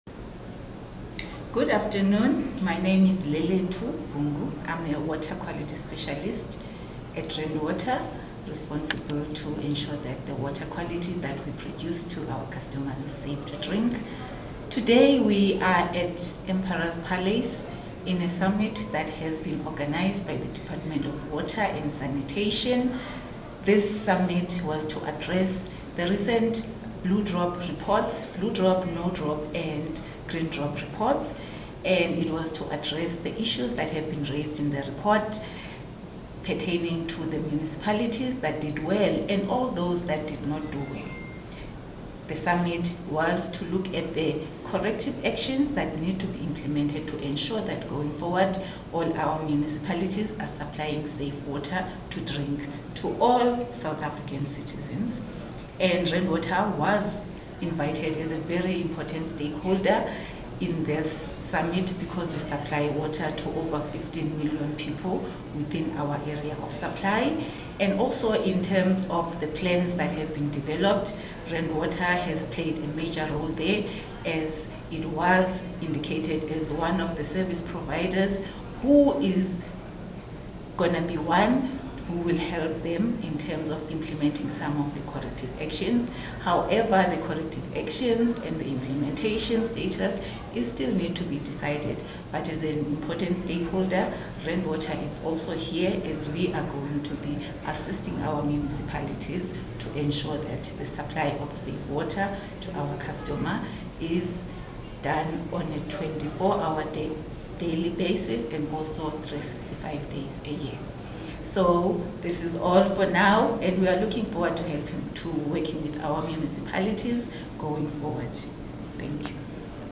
Rand Water Quality Specialist Lelethu Bungu || Water Summit 2024
Rand Water's Water Quality Specialist Lelethu Bungu speaks about the Blue Drop, No Drop, and Green Drop Reports at the WaterSummit2024 hosted by the Minister of Water and Sanitation Hon. Senzo Mchunu.